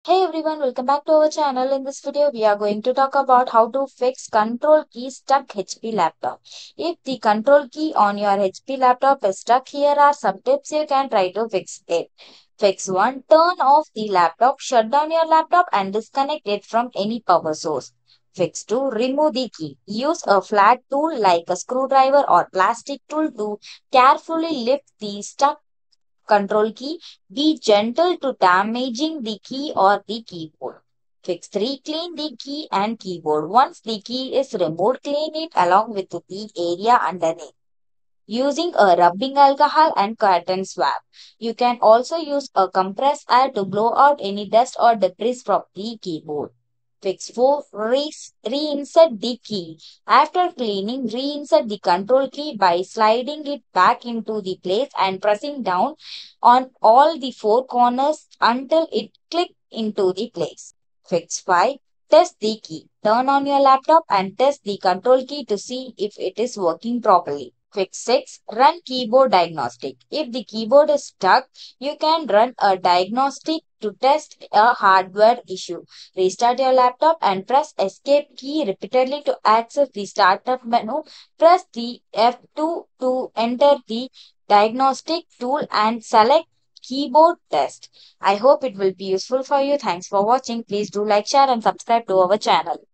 How To Fix Control Key Stuck HP Laptop
In This Informative Video, We Will Guide You Through The Process Of Fixing A Stuck Control Key On Your Hp Laptop. Whether It Is Due To Debris, Mechanical Issues, Or Software Glitches, We Will Provide Step-By-Step Instructions To Help You Resolve The Problem Effectively. Join Us As We Explore Various Troubleshooting Techniques To Restore Your Keyboard Functionality.

00:00 Introduction
00:12 Fix 1: Turn Off The Laptop
00:18 Fix 2: Remove The Key
00:31 Fix 3: Clean The Key And Keyboard
00:47 Fix 4: Reinstall The Key
01:01 Fix 5: Test The Key
01:08 Fix 6: Run Keyboard Diagnostics